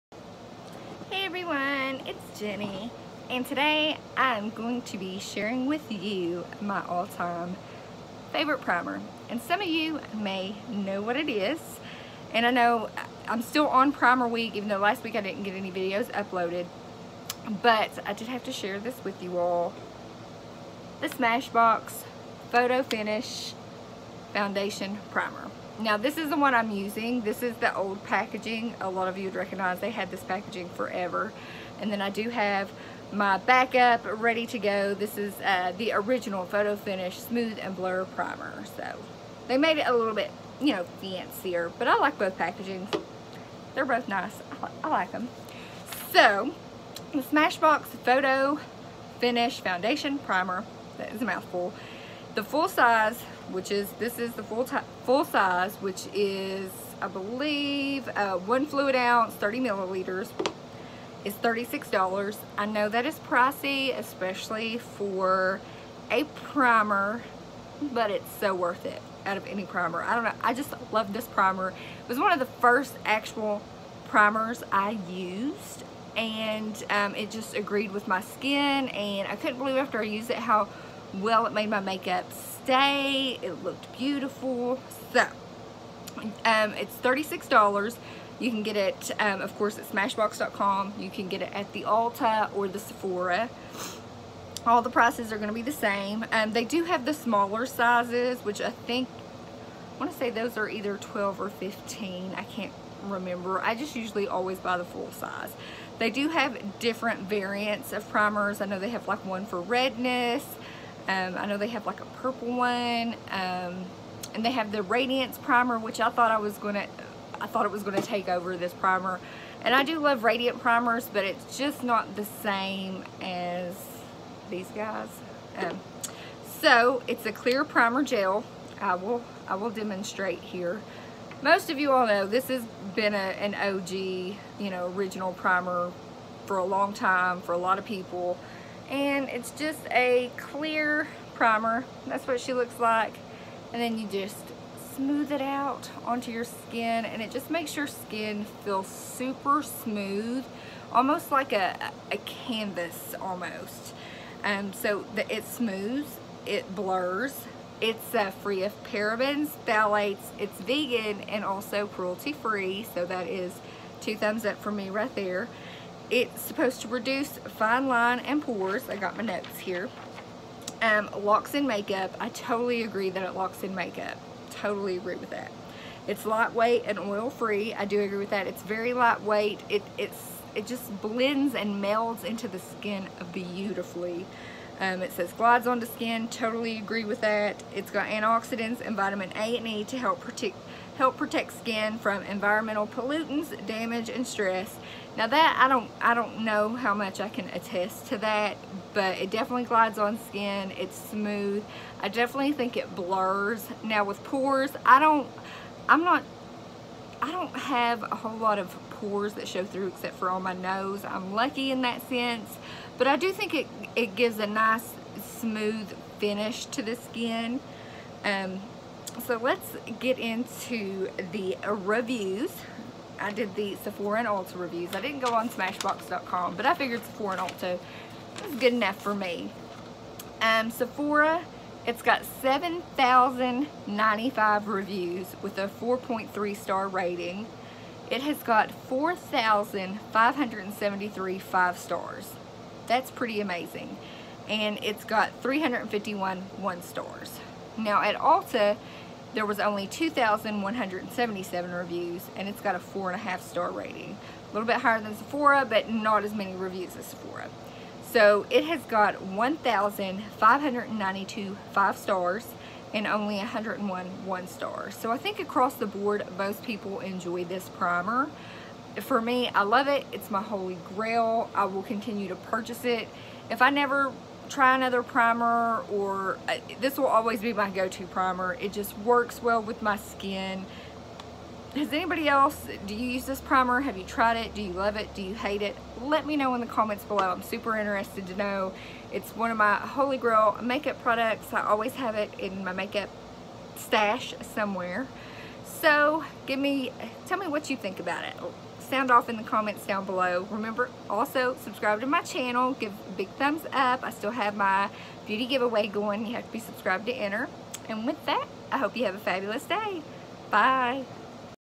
SMASHBOX PHOTO FINISH PRIMER | 5 Minute Review
Hey everyone it's Jenny and it's time for another 5 minute review and today I'm sharing with you my all time favorite primer the Smashbox Photo Finish primer. Have you tried this primer and if so what do you think about it?

Smashbox Primer ($36)

Check me out on social media (I follow back)

Walmart Beauty Box